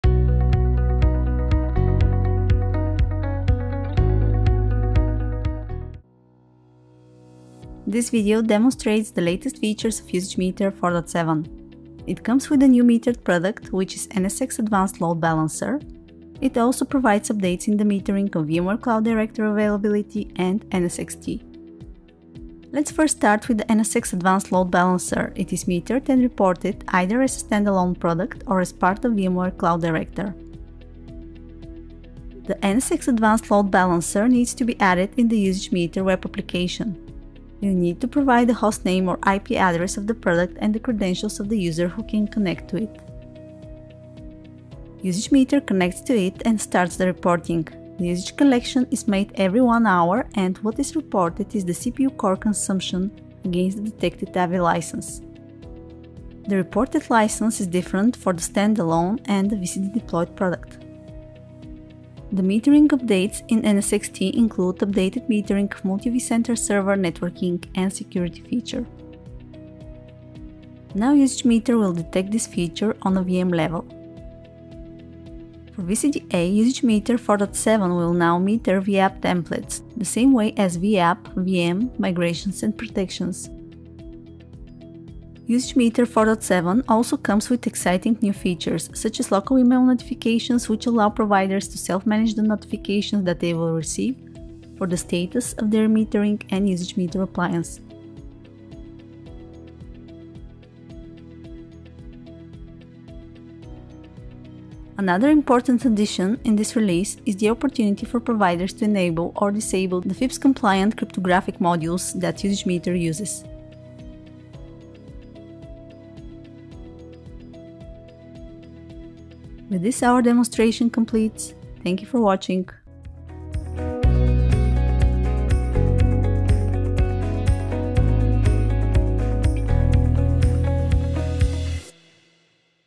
vCloud Usage Meter 4.7 Demo l VMware Cloud Services Provider
In this video, we demonstrate the latest metering and feature updates in vCloud Usage Meter 4.7